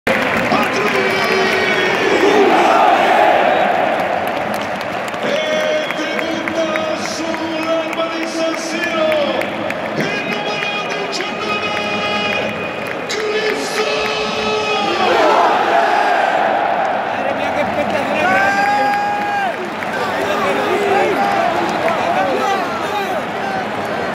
Debut de Krzyrsztof Piatek con el AC Milan
Así recibió el Estadio de San Siro al delantero polaco que aspira a volver a ilusionar a los aficionados rossoneri